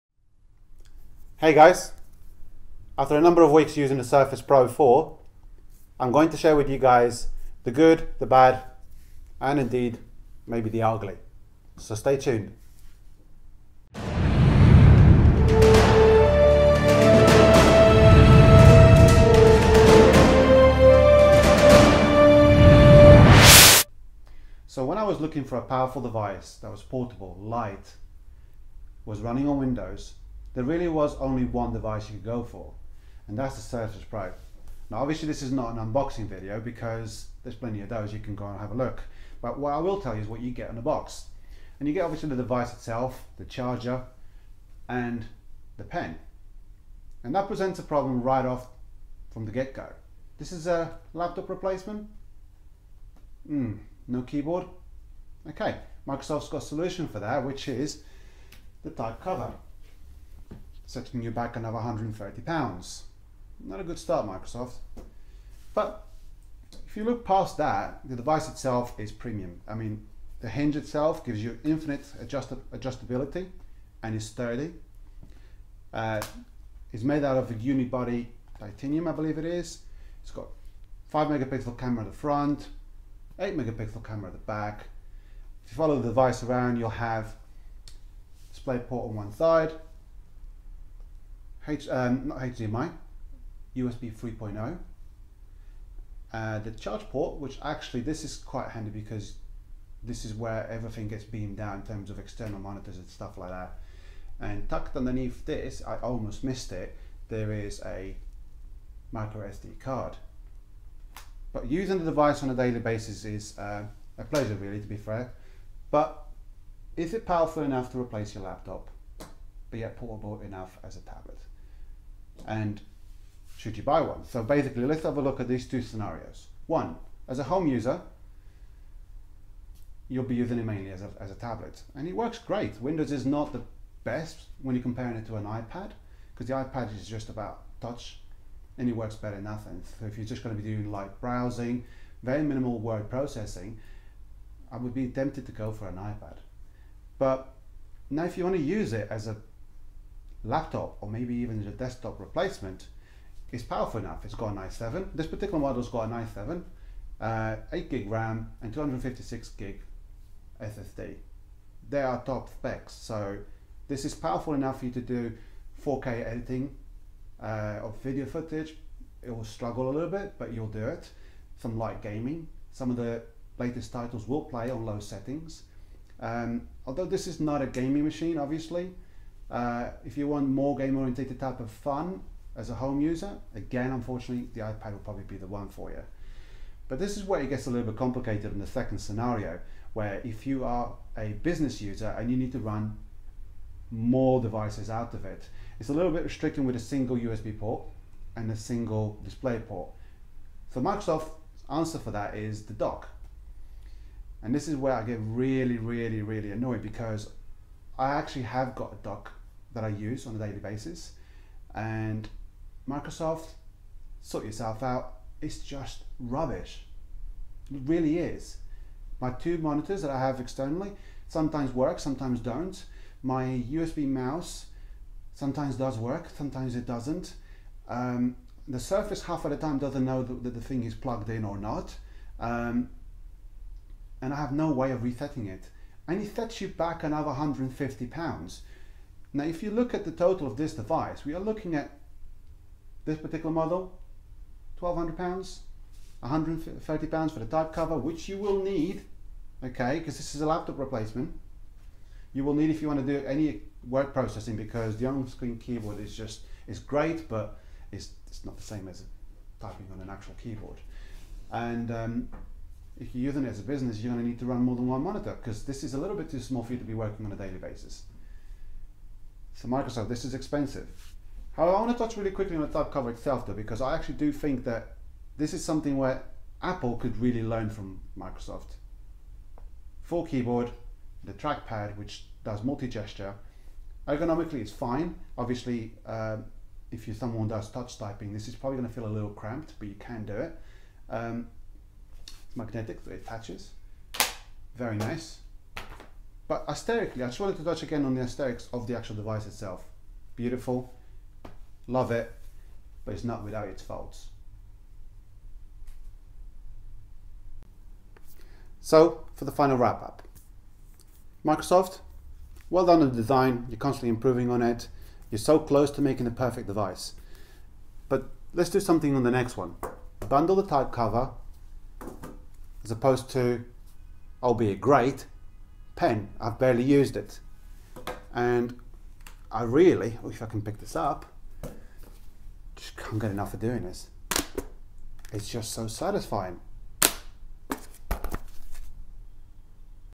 After a few weeks is it any good? (Microsoft Surface Pro 4)
My view on the latest incarnation of the Microsoft's tablet that replaces the laptop. but does it?

This video is not intended as a review but more as a personal though on the device after using for a few weeks and the shine of the new device is gone.